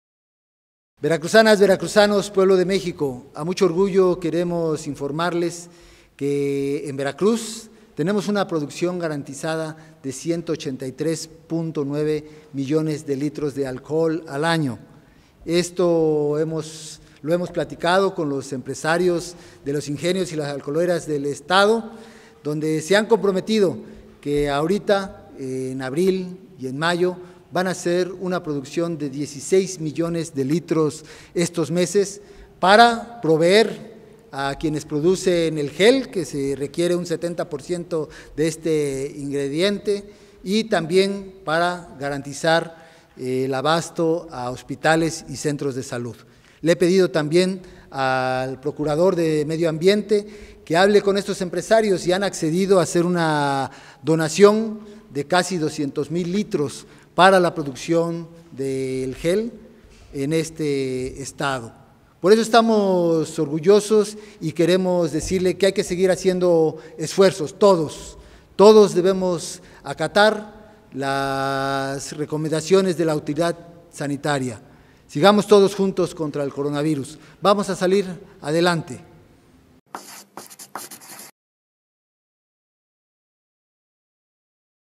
Ingenios veracruzanos cubrirán demanda de alcohol por contingencia sanitaria: Cuitláhuac García
Xalapa, Ver., 13 de abril de 2020.- El gobernador Cuitláhuac García Jiménez dio a conocer que Veracruz tiene una producción anual garantizada de 183.9 millones de litros de alcohol, de forma que puede cubrir la demanda por la contingencia sanitaria que enfrenta el país, principalmente durante abril y mayo.

En este sentido, destacó el compromiso de empresarios de la industria para obtener 16 millones mensuales, a fin de proveer a quienes elaboran gel antibacterial y abastecer a hospitales y centros de salud.

La producción se distribuye en los ingenios San José de Abajo (Cuitláhuac), con un millón 450 mil litros, y La Gloria (Úrsulo Galván), 36.1 millones; así como Grupo Báltico (Orizaba), 46 millones; Central Energética de Atoyac, 17.5; Destilería Nopaltepec (Cosamaloapan), 28.8, y Destiladora de Mieles (Cuitláhuac), 54 millones.